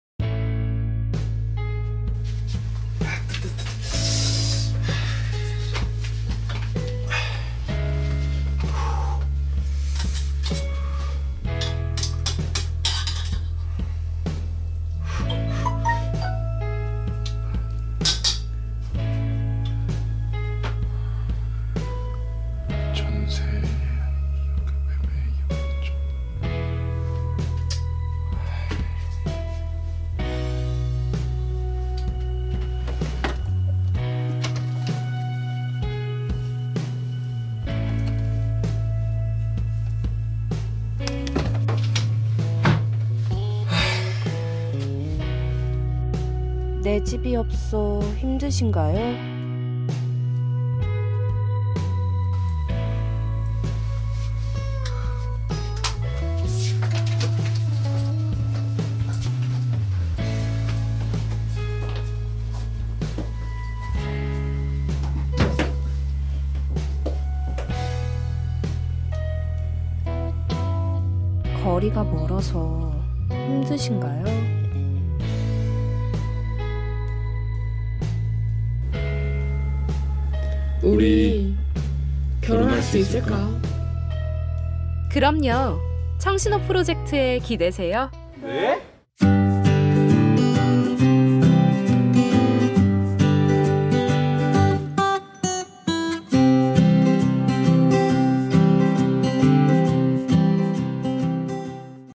청신호 UCC 웹툰 공모전 동상 '우리 결혼할 수 있을까?'
여자친구에게 프로포즈를 하고 싶으나
가장 큰 문제인 집구하기에 어려움을 겪고 있는 ‘승훈’.

매일매일 긴 출퇴근 시간으로 바쁘고 여유가 없는 ‘현성’.

이들이 마음껏 사랑하고
미래를 꿈꿀 수 있도록
‘내 집’ 마련을 위한 청신호 프로젝트가 다가온다!